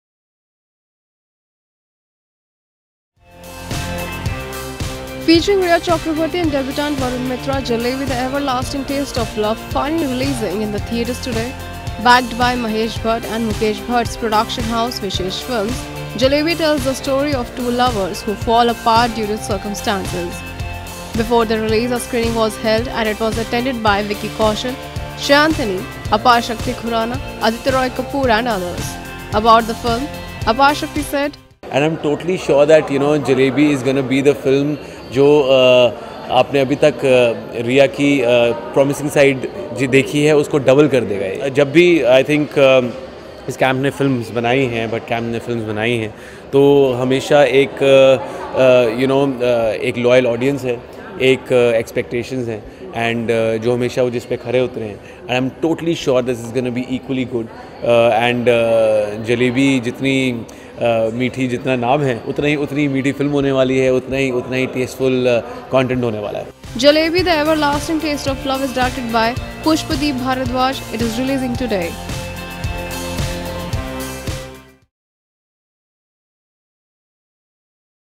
Jalebi SCREENING | Vicky, Aditya Looks EXCITED
Featuring Rhea Chakraborty and debutant Varun Mitra, ‘Jalebi- The Everlasting Taste Of Love’ finally releasing in the theatres today. Backed by Mahesh Bhatt and Mukesh Bhatt’s Production house Vishesh Films “Jalebi” tells the story of two lovers who fall apart due to circumstances.  A screening was held and it was attended by, Vicky Kaushal, Sayantani, Aparshakti Khurrana, Aditya Roy Kapur and others.